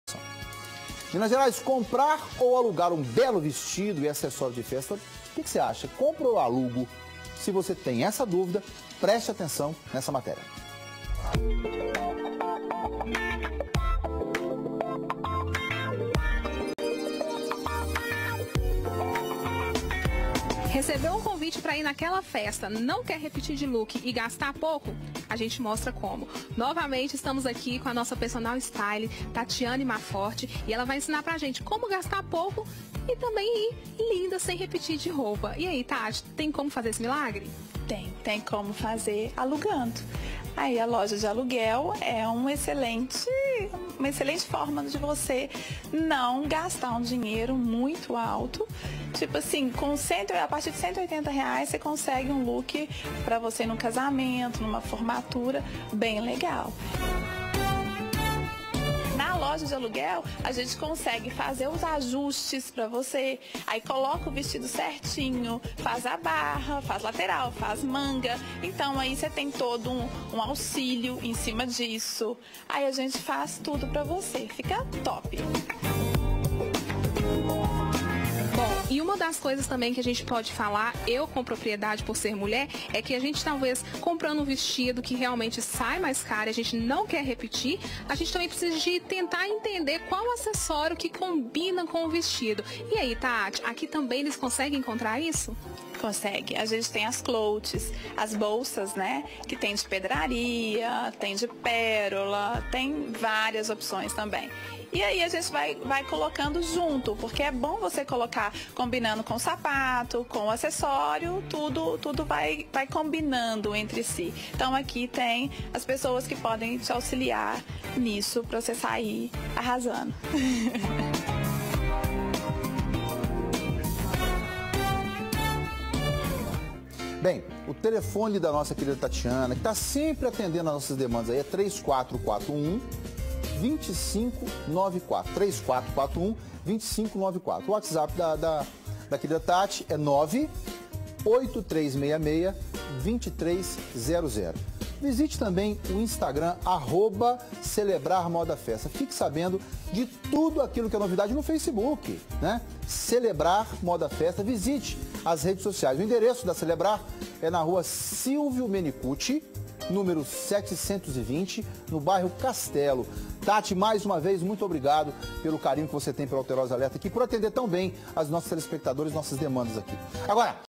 Comprar ou alugar roupas de festa?
Bailes de formatura chegando, vários casamentos a vista e você ainda está com dúvidas se aluga ou compra aquele vestido de festa pra arrasar na ocasião. Confira as dicas e resolva o problema.